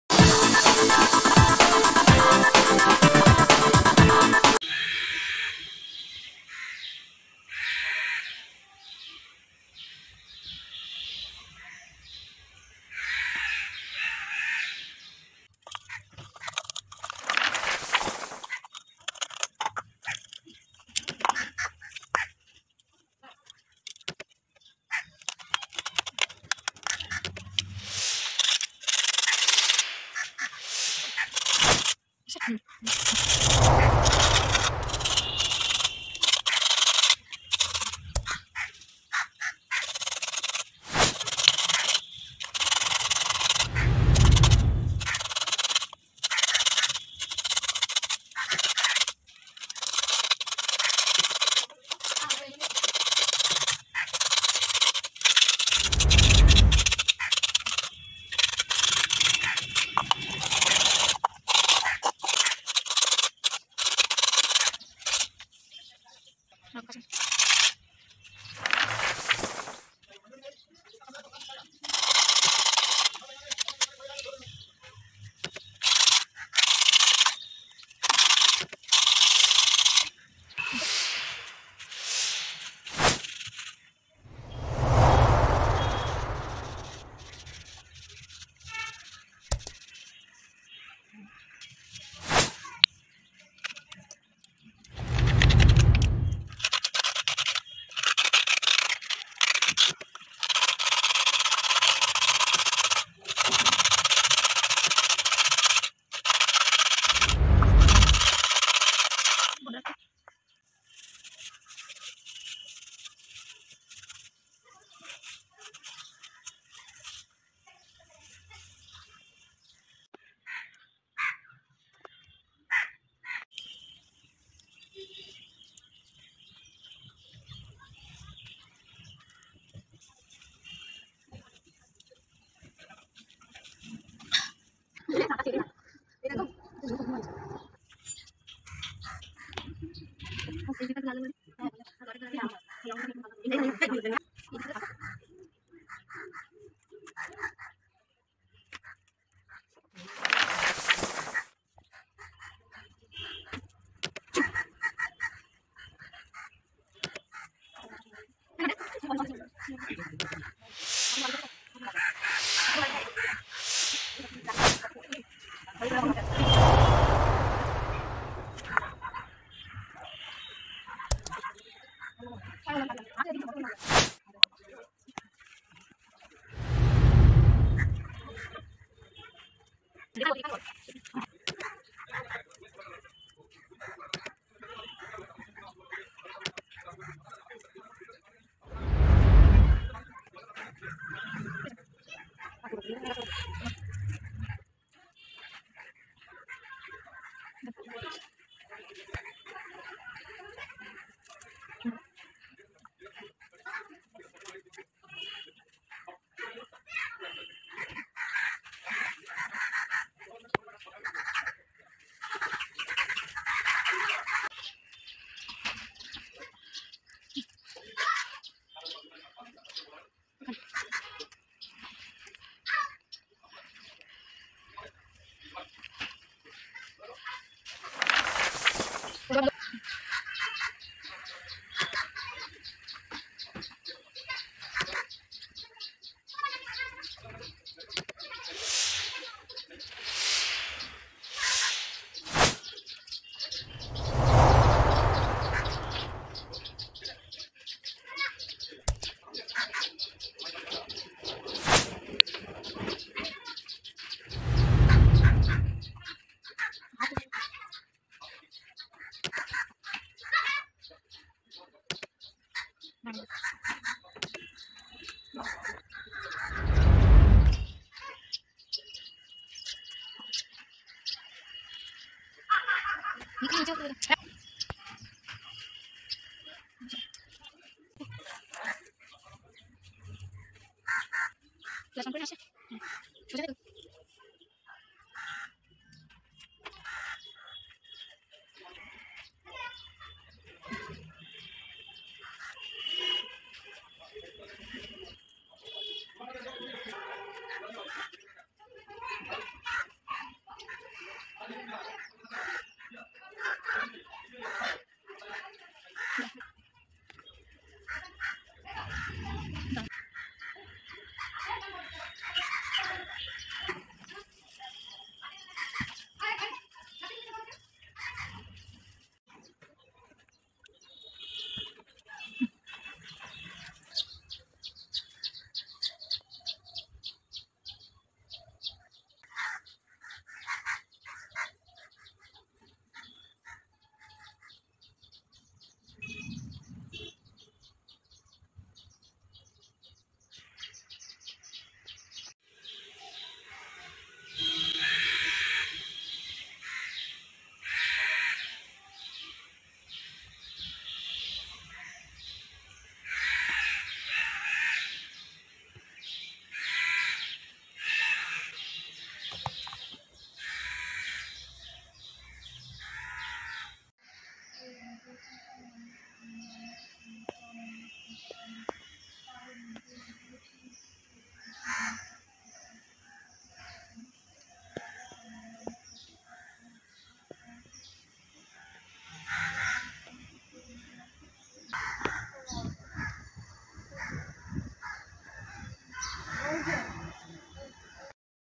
coconut shell wall hanging || coconut shell craft ideas,coconut shell craft
by _ sucharita Haldar

Coconut shell into a beautiful hanging || how to craft coconut shell into a beautiful hanging